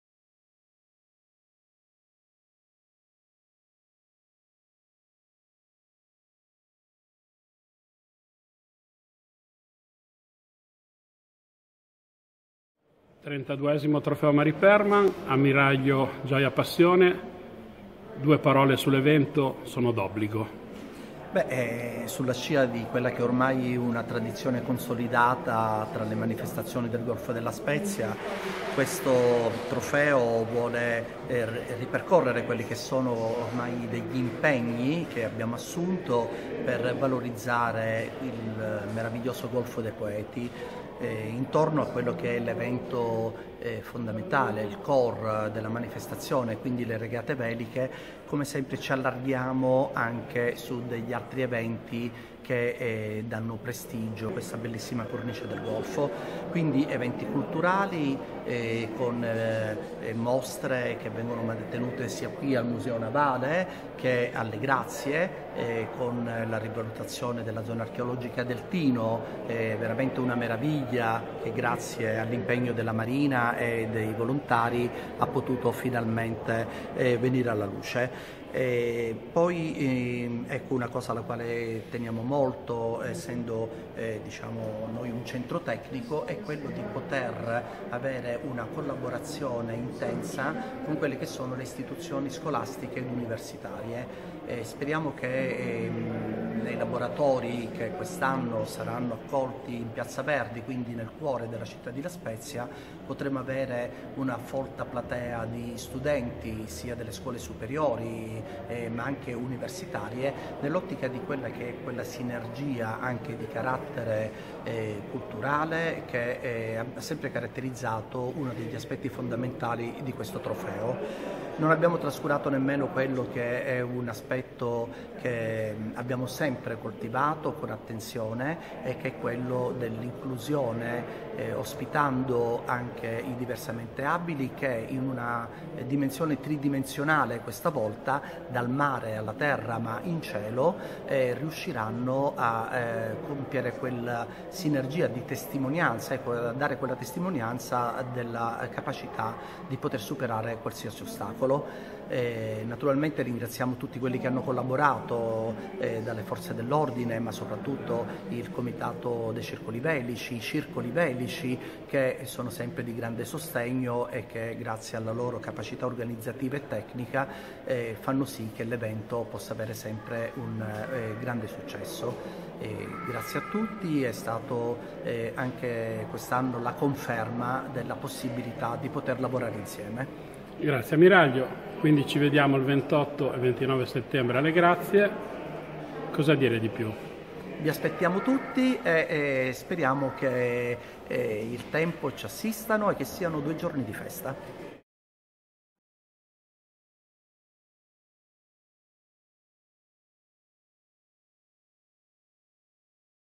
Intervista all'Ammiraglio Ispettore Rosario Gioia Passione
32° Trofeo Mariperman
In occasione della conferenza stampa di presentazione del 32° Trofeo Mariperman, che si svolgerà a Le Grazie di Porto Venere il 28-29 ottobre 2019, abbiamo intervistato l'Ammiraglio Ispettore Rosario Gioia Passione Direttore del CSSN Centro di Supporto e Sperimentazione Navale della Marina Militare di La Spezia.